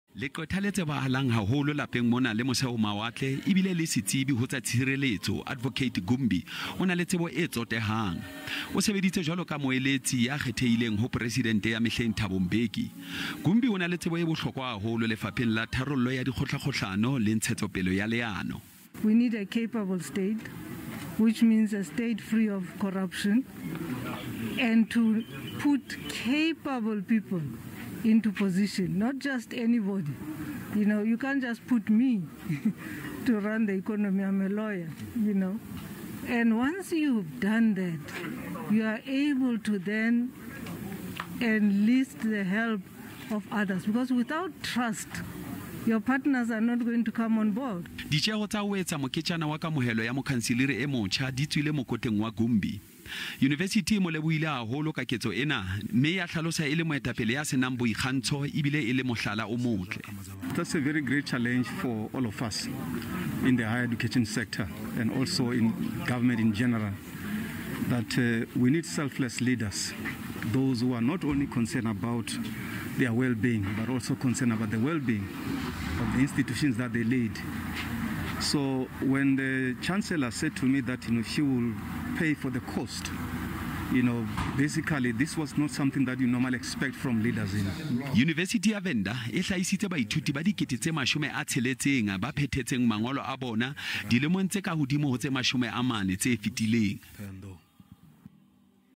Adv. Mojanku Gumbi ke mokhanseliri emotjha wa Univesithi ya Venda
Leqwetha la ditokelo tsa botho advocate Mojanku Gumbi le re Afrika Borwa e hloka baetapele ba rutehileng ba bileng ba ena le maitshwaro a matle, ho lwantsha bobodu le puso e mpe. Gumbi o buile tjena nakong ya tlhomamiso ya hae e le mokanseliri wa pele wa mosadi Univesithing ya Venda, Limpopo.

For more news, visit sabcnews.com and also SABCNews SABCDitaba COVID19News on Social Media.